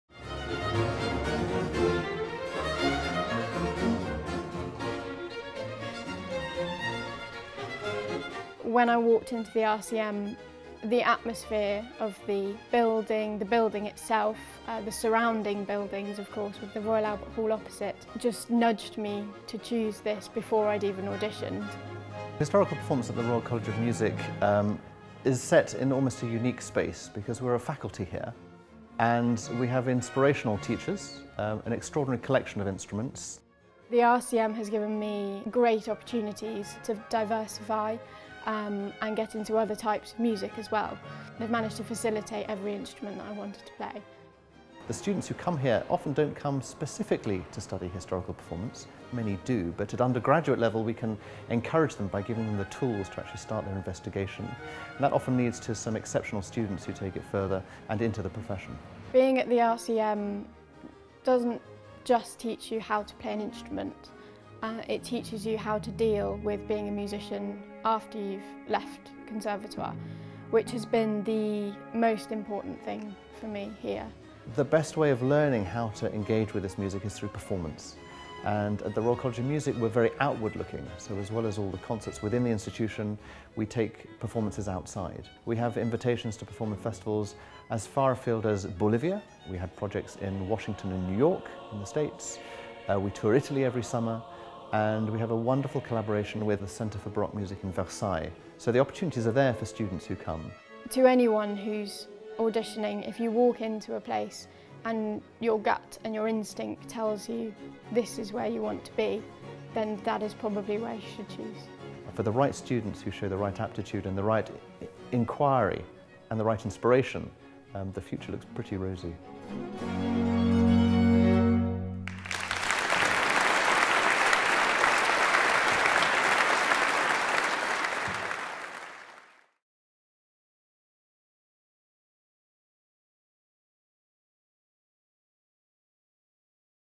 Historical Performance at the Royal College of Music
Head of Historical Performance, Professor Ashley Solomon, joins recorder and historical oboe player Katie Cowling to talk about the opportunities RCM students enjoy.

Find out more about the Royal College of Music's Historical Performance Faculty